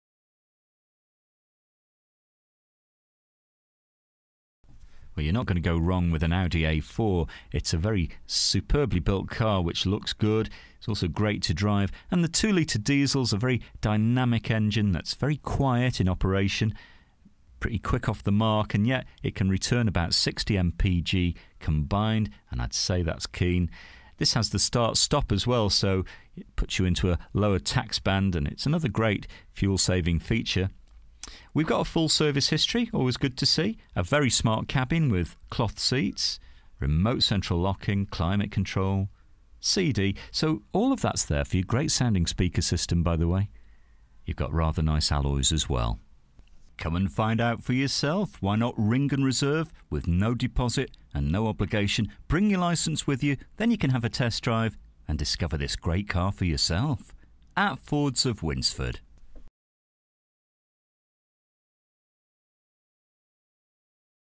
AUDI A4 DIESEL SALOON (2010) 2.0 TDIE 136 SE 4DR [START STOP] - GV10BXS
DETAILS:
First registered: 28/05/2010
Current mileage: 51100
Registration: GV10BXS
Colour: BLACK

FEATURES:
full service history, all round electric windows, remote central locking, cloth interior, all round airbags, climate control, pas, abs, alloy wheels, solid paint, cd, traction control, parking control, hand book present, cruise contro

This used car is for sale at Fords of Winsford Car Supermarket in Cheshire, UK